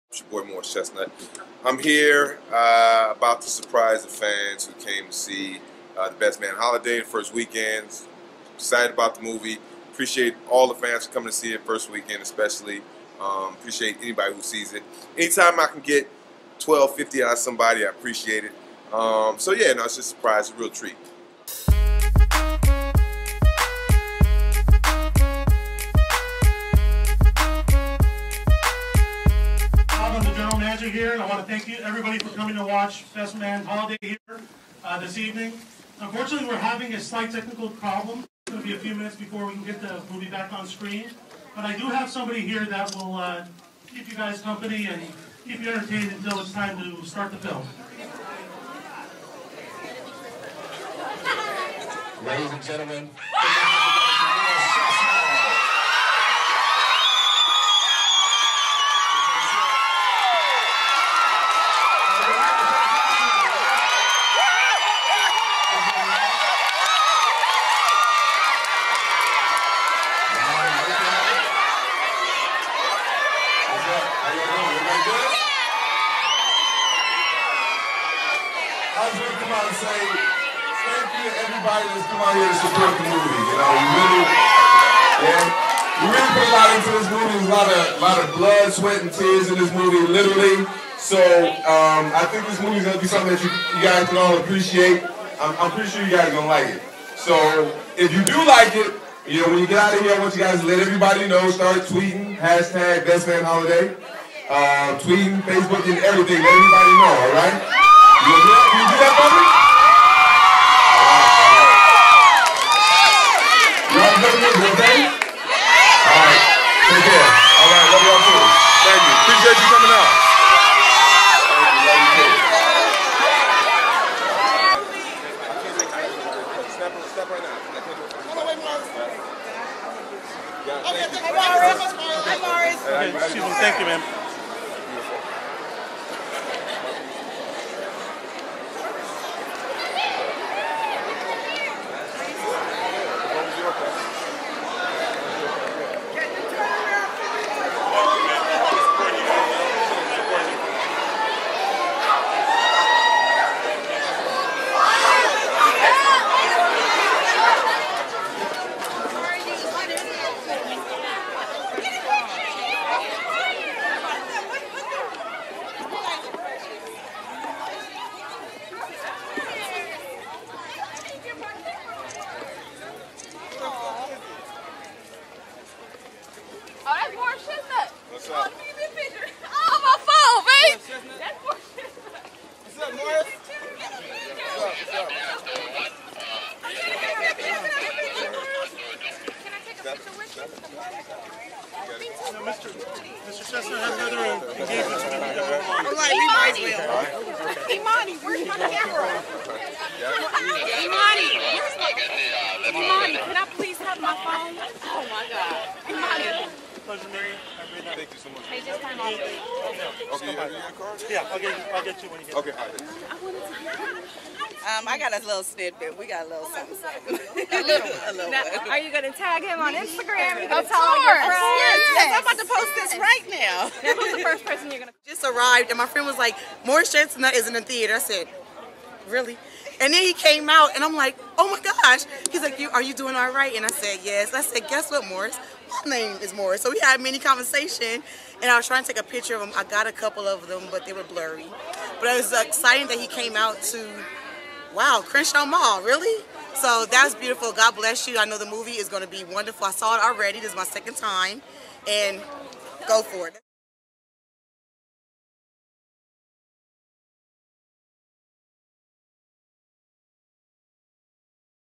Morris Chestnut Surprises Theater Goers
Best Man Holiday actor made a surprise appearance at the Rave Crenshaw theater in Baldwin Hills to thank his fans for coming out to support the film.